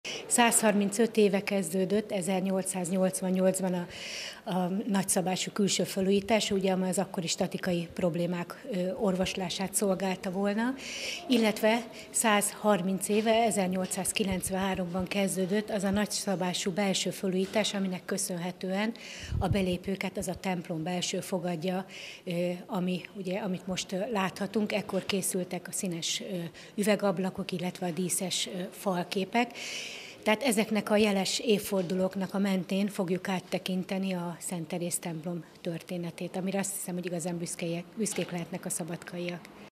2023.05.17. - Építéstörténeti utazás
Lakatos Adél a szabadkai Szent Teréz székesegyház jeles évfordulóira tért ki előadásában. Elmondta, hogy Szabadka kiemelkedő épülete idén 250 éves, de nemcsak ezt a kerek évfordulót ünnepli. A programot a múzeumok hete rendezvénysorozat keretében tartották meg.